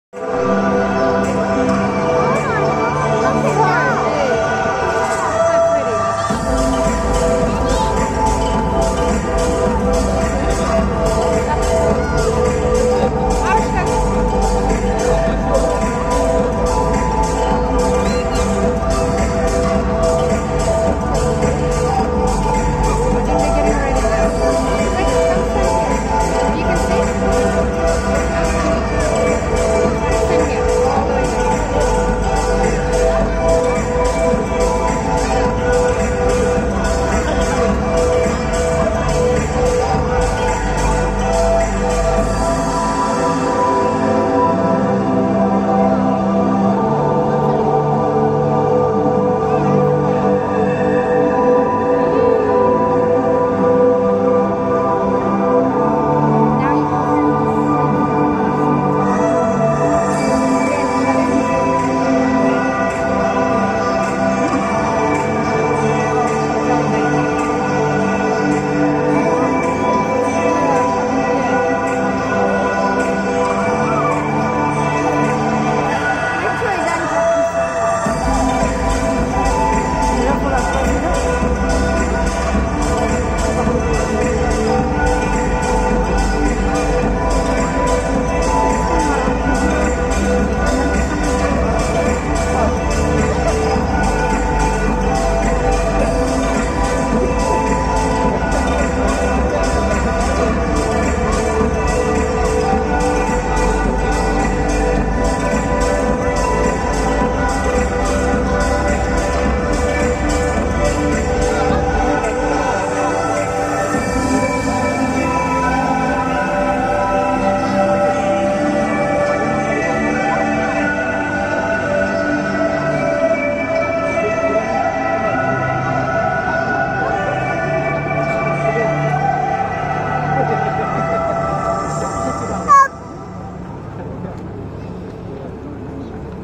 BURJ KHALIFA LIGHTNING SHOW
Burj Khalifa is the building which needs no description around the world. To be there and experience that is a different and awesome feeling, but guess what's better than that is to just watch the lighting show of burj khalifa, and that was too beautiful.